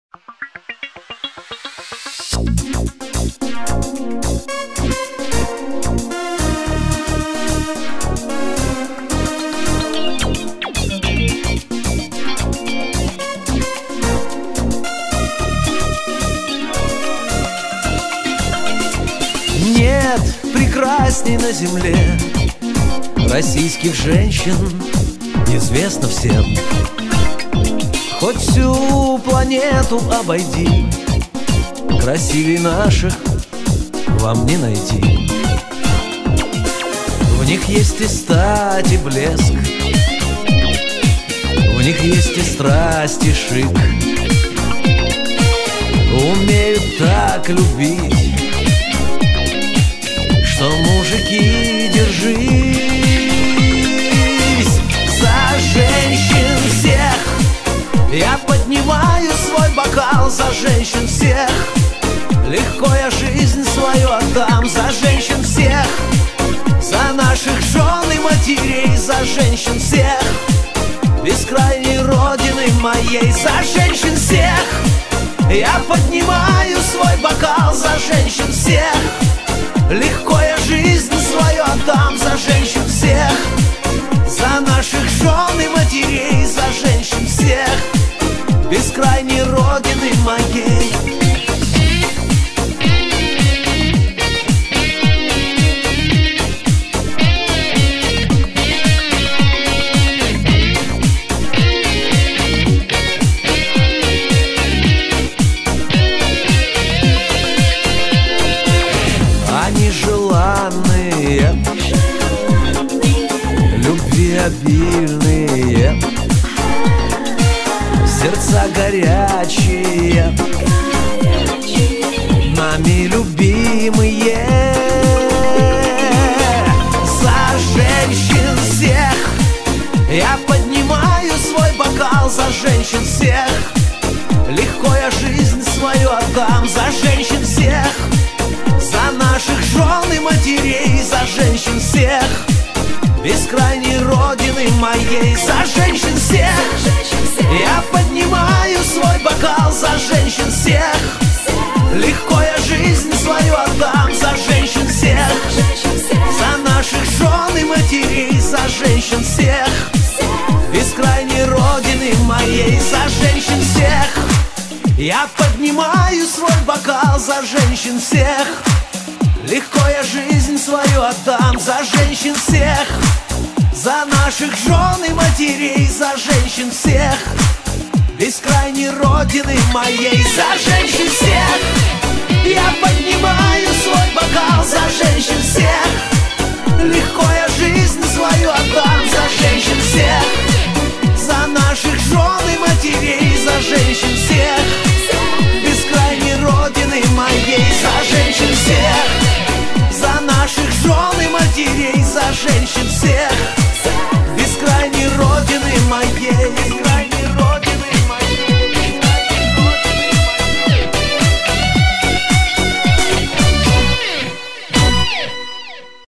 Поздравление с женским праздником 8 марта (08.03.21)
Светлое и нежное видеопоздравление для женщин в чудесный праздник 8 марта. Создано на основе красочных открыток, стихов с наилучшими и позитивными пожеланиями. Сочетается с зажигательной и яркой песней, посвященной женщинам.